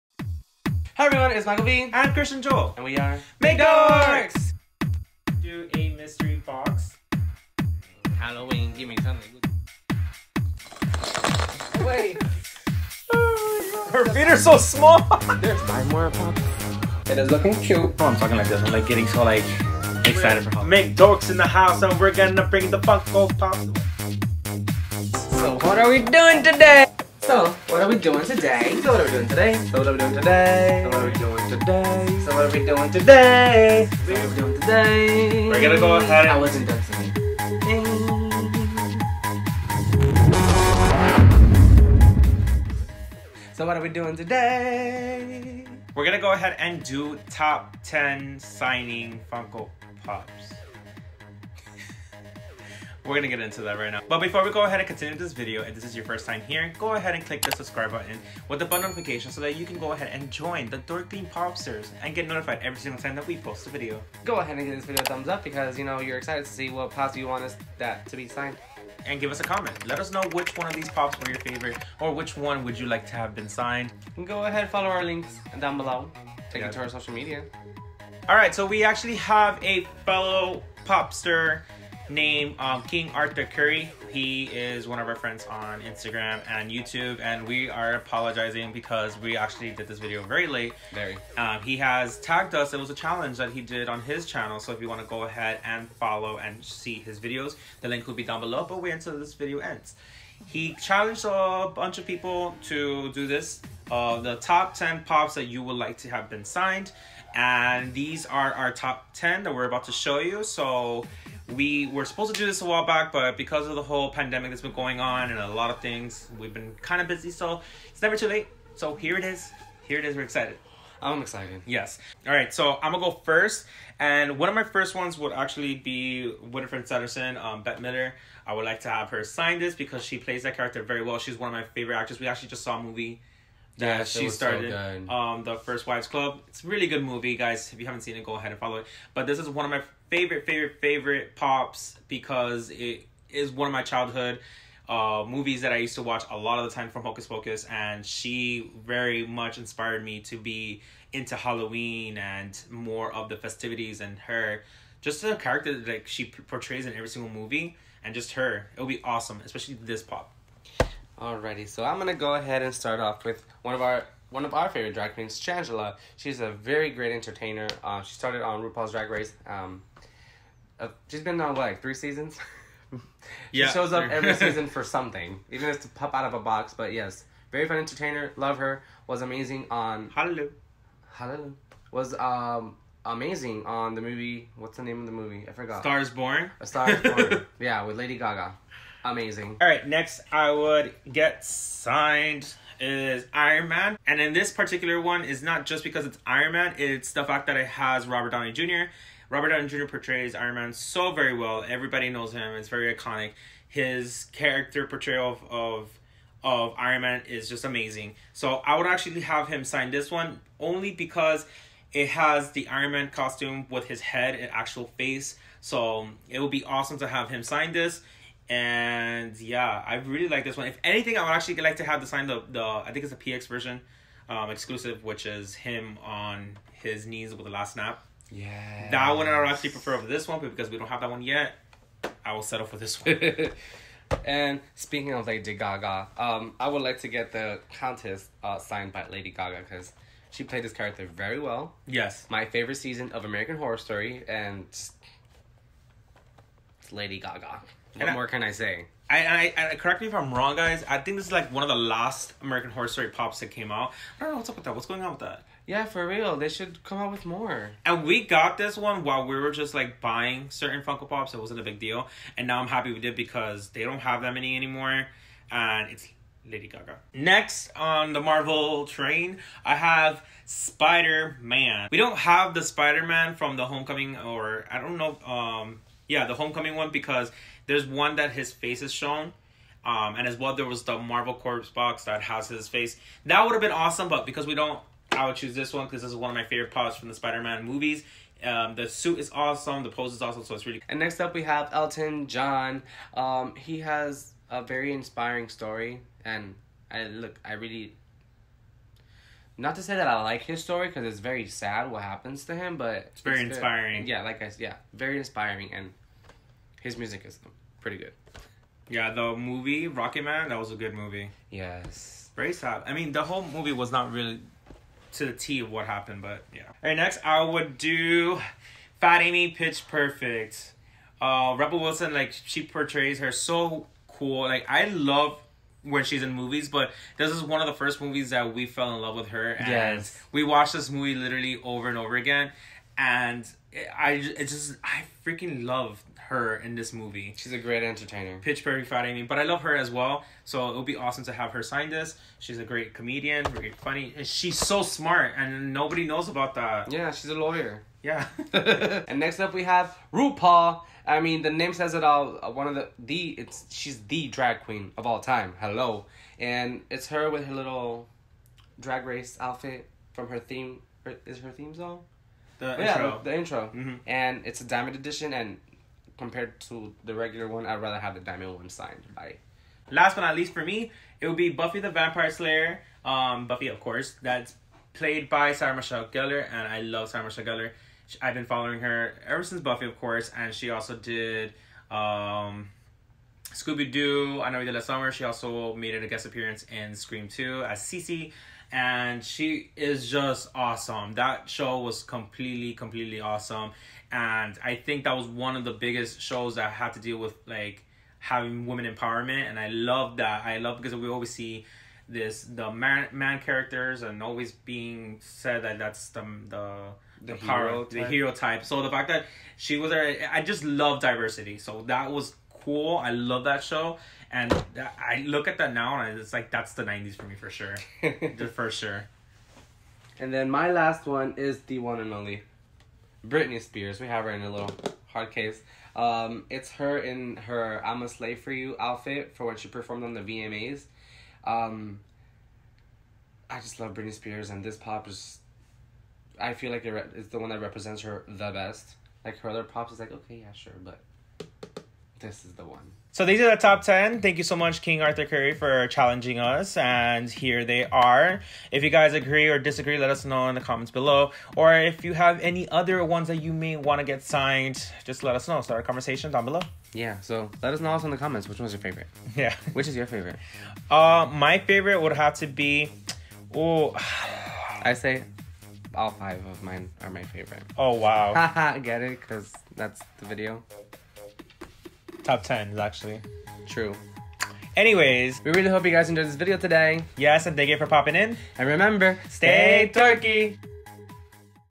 Top 10 Autographed Funko Pops Challenge
We got challenged by King Arthur Curry to pick out Top 10 funko pops that we which we were autographed.